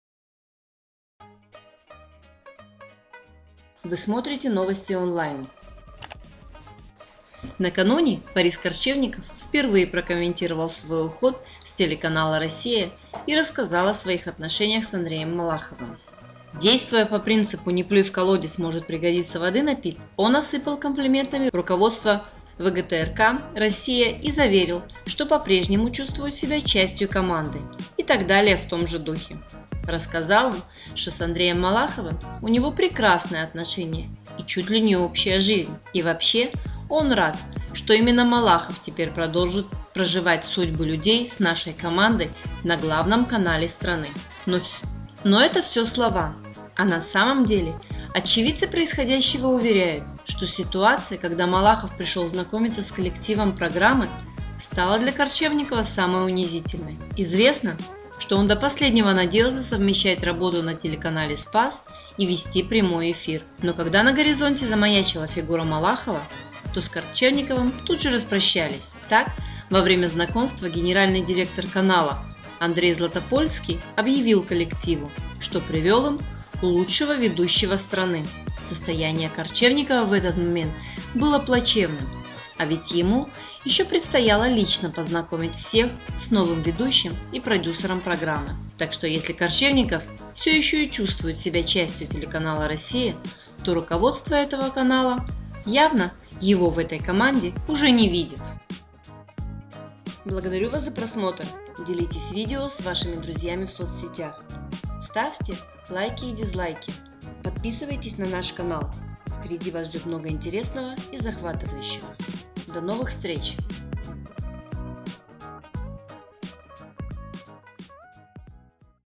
БОРИС КОРЧЕВНИКОВ ЗАМЯЛ ИСТОРИЮ СВОЕГО ПУБЛИЧНОГО УНИЖЕНИЯ (21.08.2017)
БОРИС_КОРЧЕВНИКОВ ЗАМЯЛ ИСТОРИЮ СВОЕГО ПУБЛИЧНОГО УНИЖЕНИЯ
Накануне БорисКорчевников впервые прокомментировал свой уход с телеканала Россия и рассказал об отношениях с Андреем Малаховым.
Действую по принципу «не плюй в колодец – может пригодится, воды напиться», он осыпал комплиментами руководство ВГТРК Россия и заверил, что по прежнему чувствует себя частью команды, и так далее в том же духе.

Рассказал он, что с Андрем Малаховым у него прекрасные отношения и чуть ли не общая жизнь. И вообще он рад, что именно Малахов  «теперь продолжит проживать судьбы людей  с нашей командой на главном канале страны».